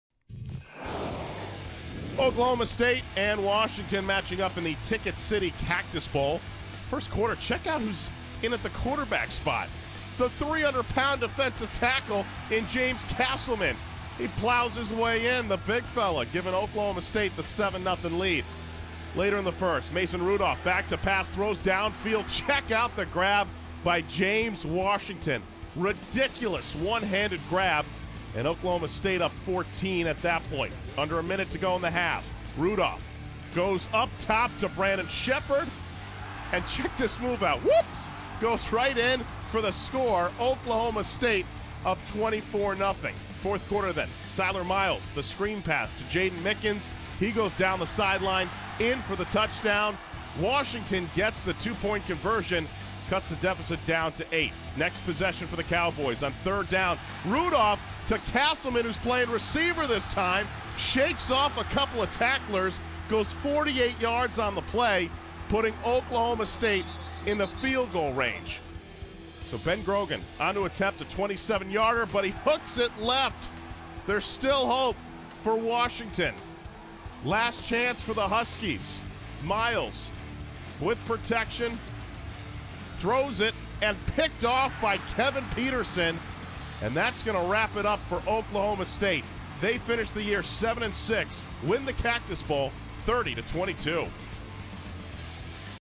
Oklahoma State Wins TicketCity Cactus Bowl
Oklahoma State held off Washington's late rally to beat the Huskies 30-22 in the TicketCity Cactus Bowl.